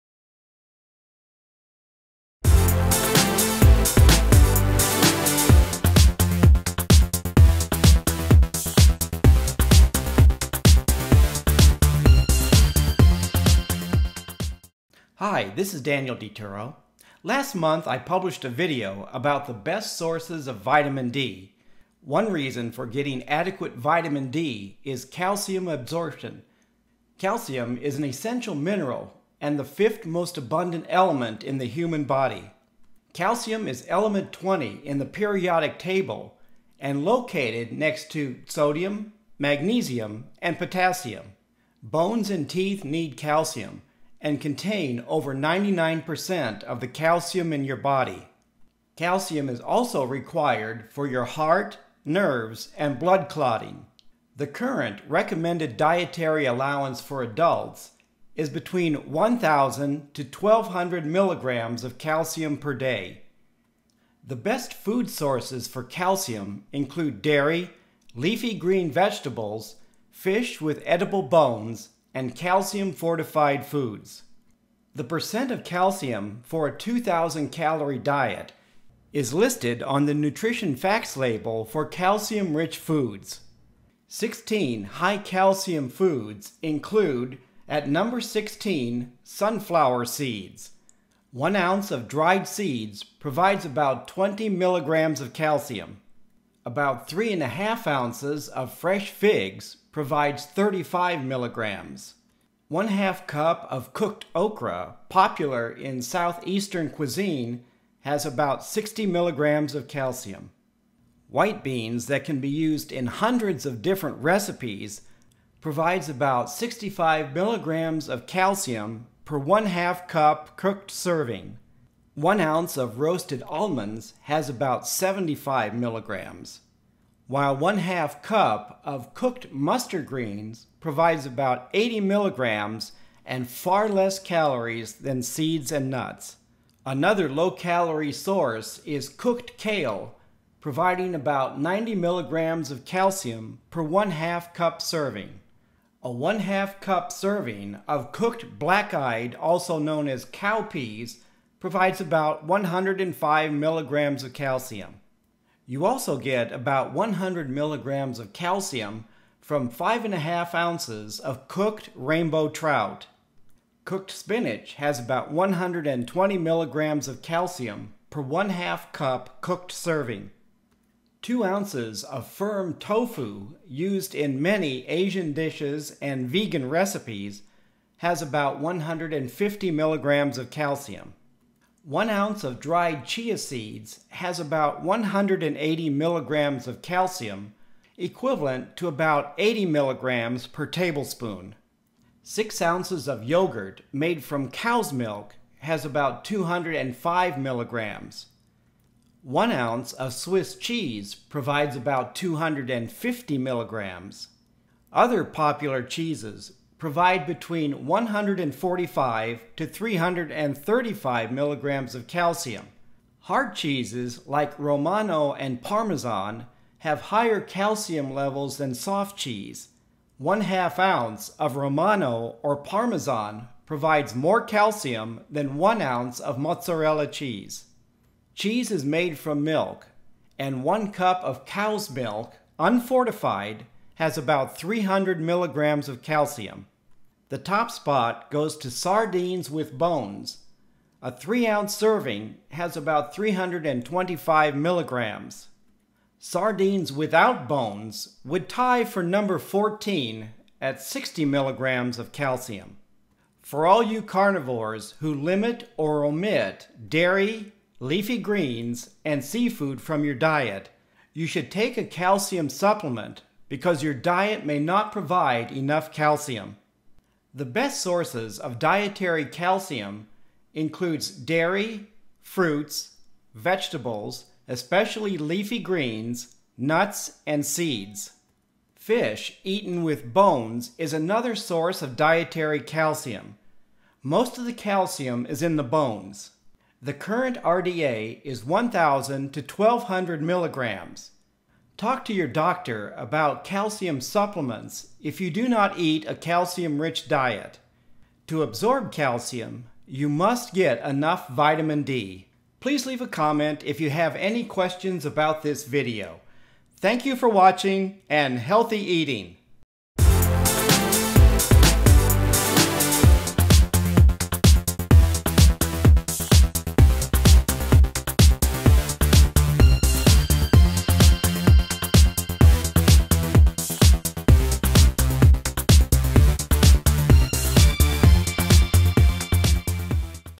16 High Calcium Foods (700 Calorie Meals) DiTuro Productions
16 common high calcium foods, why calcium is important, and the USRDA for calcium.